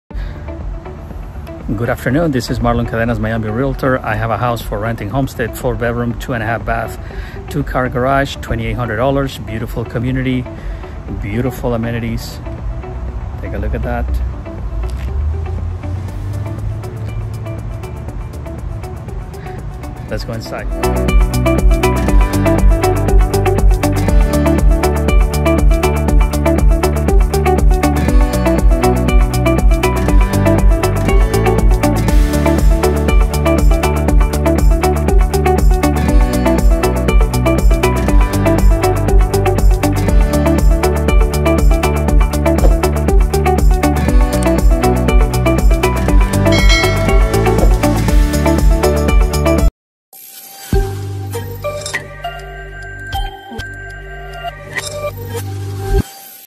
House for rent in Homestead FL
This house is rented for $2,800. The house has 4 bedrooms and 2.5 bathrooms, and a double garage.

Great community with great amenities. Great location in Homestead close to the air base, schools, restaurants, shopping center, and the highway.